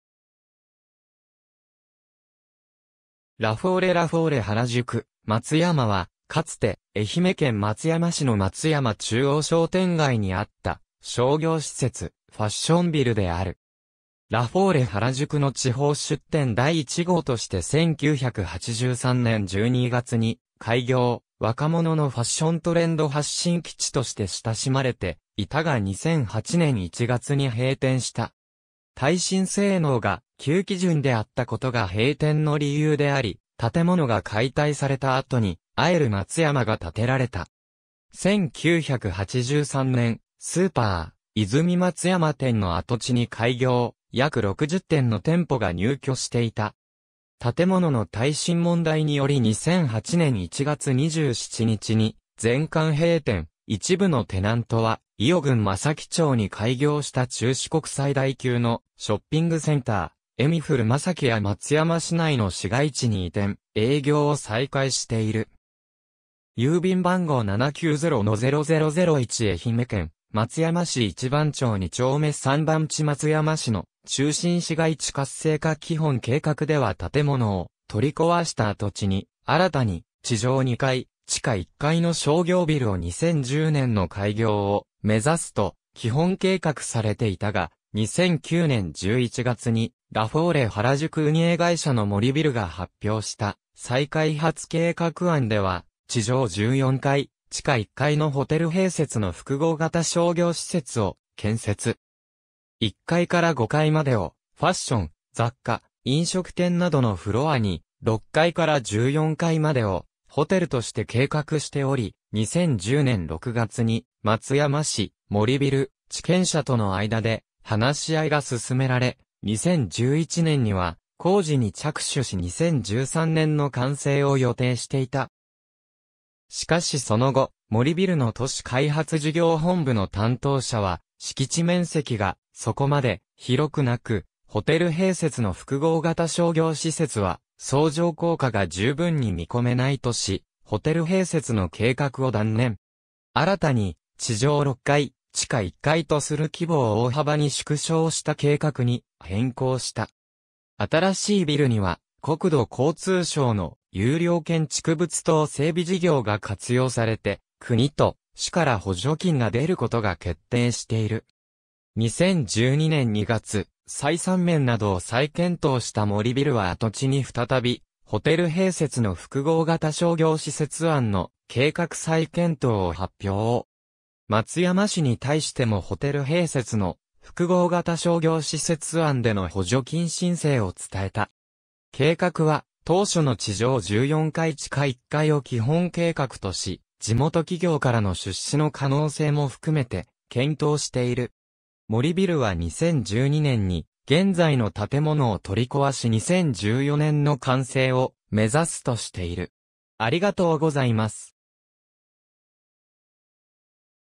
ラフォーレ原宿・松山
ラフォーレ ラフォーレ原宿・松山（ラフォーレはらじゅく・まつやま）は、かつて愛媛県松山市の松山中央商店街にあった商業施設、ファッションビルである。
ラフォーレ原宿（東京・表参道）の地方出店第1号として1983年（昭和58年）12月に開業、若者のファッショントレンド発信基地として親しまれていたが2008年（平成20年）1月に閉店した。
耐震性能が旧基準であったことが閉店の理由であり、建物が解体された後にAEL MATSUYAMAが建てられた。
1983年（昭和58年）、スーパーいづみ松山店の跡地に開業、約60店の店舗が入居していた。
建物の耐震問題により2008年（平成20年）1月27日に全館閉店、一部のテナントは伊予郡松前町に開業した中四国最大級のショッピングセンター、エミフルMASAKIや松山市内の市街地に移転・営業を再開している。
〒790-0001 愛媛県松山市一番町二丁目3番地 松山市の中心市街地活性化基本計画では建物を取り壊した跡地に、新たに地上2階、地下1階の商業ビルを2010年の開業を目指すと基本計画されていたが、2009年11月にラフォーレ原宿運営会社の森ビルが発表した再開発計画案では地上14階、地下1階のホテル併設の複合型商業施設を建設。
1階から5階までをファッション、雑貨、飲食店などのフロアに、6階から14階までをホテルとして計画しており、2010年6月に松山市、森ビル、地権者との間で話し合いが進められ、2011年には工事に着手し2013年の完成を予定していた。
しかしその後、森ビルの都市開発事業本部の担当者は「敷地面積がそこまで広くなく、ホテル併設の複合型商業施設は相乗効果が十分に見込めない」とし、ホテル併設の計画を断念。
新たに地上6階、地下1階とする規模を大幅に縮小した計画に変更した。
新しいビルには国土交通省の「優良建築物等整備事業」が活用されて国と市から補助金が出ることが決定している。
2012年2月、採算面などを再検討した森ビルは跡地に再びホテル併設の複合型商業施設案の計画再検討を発表。
松山市に対してもホテル併設の複合型商業施設案での補助金申請を伝えた。
計画は当初の地上14階地下1階を基本計画とし地元企業からの出資の可能性も含めて検討している。
森ビルは2012年に現在の建物を取り壊し2014年の完成を目指すとしている。